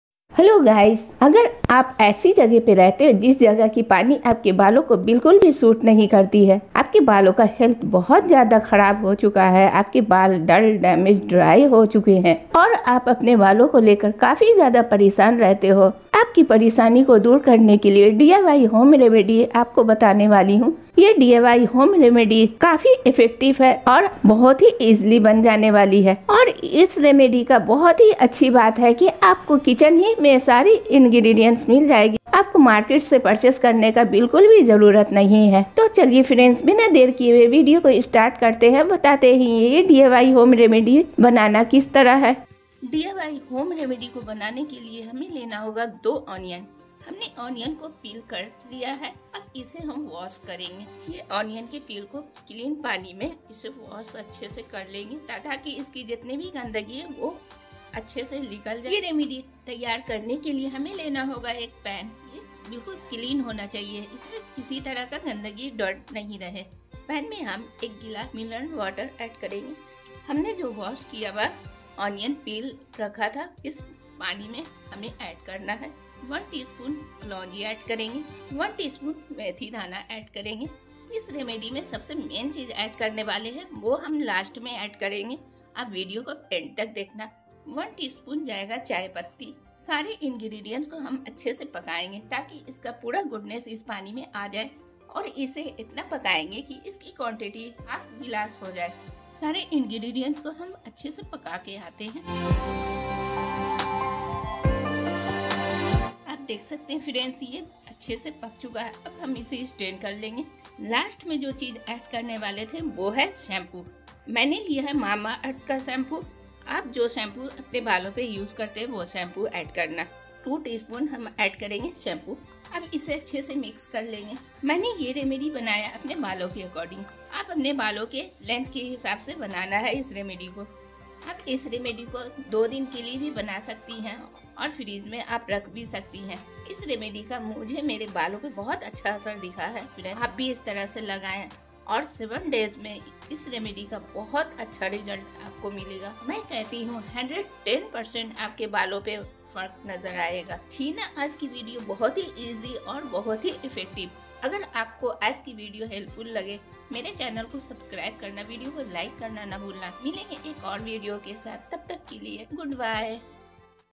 How to cure dull hair | Dull hair को कैसे ठीक करें | DIY Dull Hair Home Remedy
दोस्तों आप लोगों के बाल बहुत ही ज्यादा Dull हो गये हैं या फिर बहुत ही ज्यादा झड़ रहे हैं तो इस video को अंत तक देखना क्युकी इस video में मैंने इसी का DIY home remedy बताया है जिस से आपलोग अपने बलों की सही तरीके से देख-भाल कर सकें।